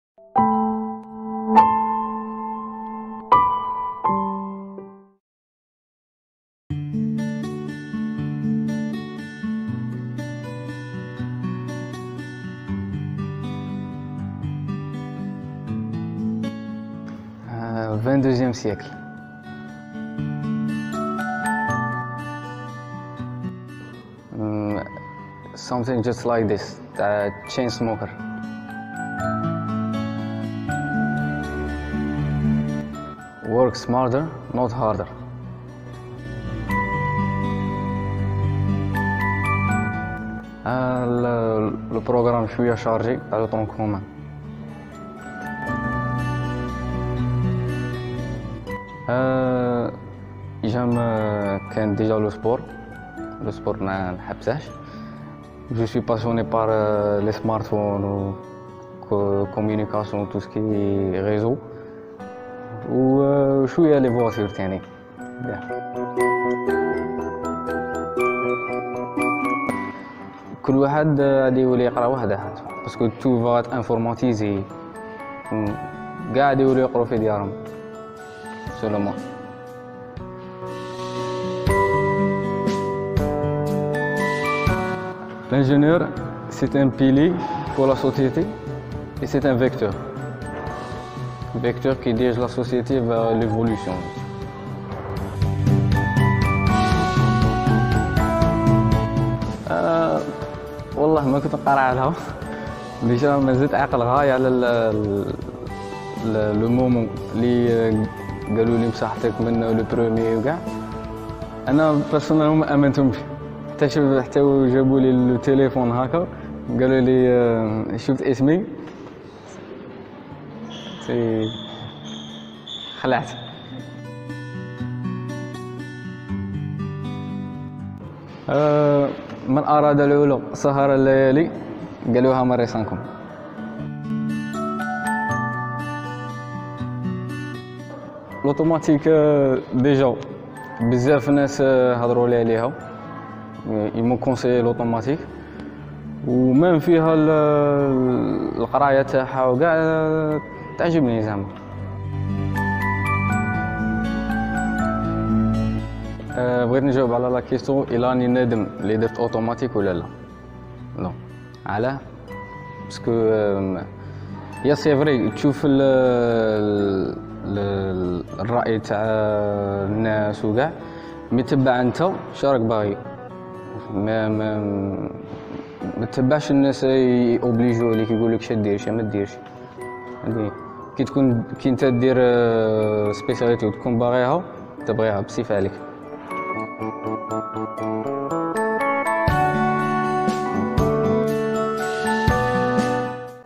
Interview de Mohammed Djemai - Majorant du concours 2019 à l'ENPO-MA | InterVium
L'interview de Mohammed Djemai, majorant du concours d'accès aux grandes écoles 2019 à l'Ecole Nationale Polytechnique d'Oran, Maurice Audin (ENPO-MA), il était étudiant en classes préparatoires à l'Ecole Supérieure de Génie Electrique et Énergétique (ESGEE).